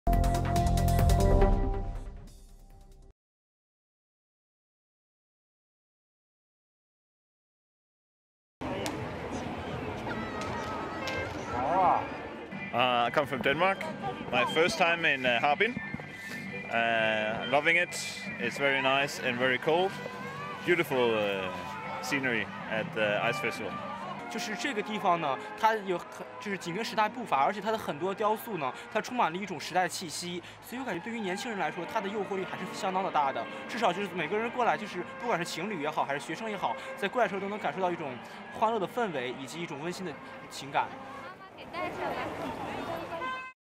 China Ice Festival Video | Ice and Snow Structures
More than 10,000 workers built ice and snow structures in just 15 days as part of the Harbin International Ice and Snow Festival. Tourists from around the world braved the cold to experience the festival. (Photo/Video: AP)